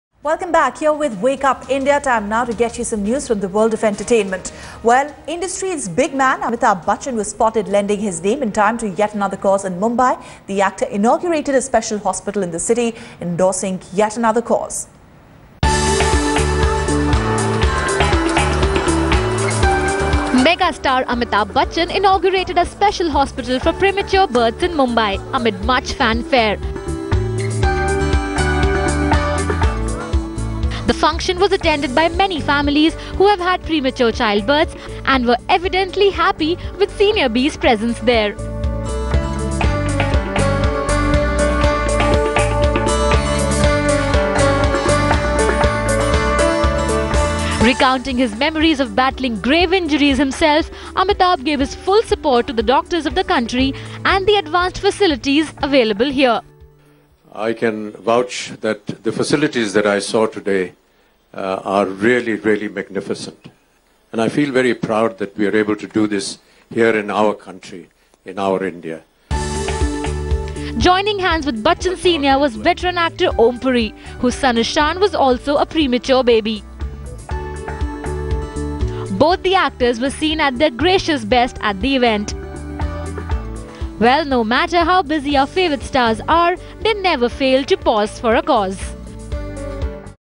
Amithabh Bachchan inaugurates hospital for special cause
The hospital he opened specialises in taking care of premature babies and for their mothers.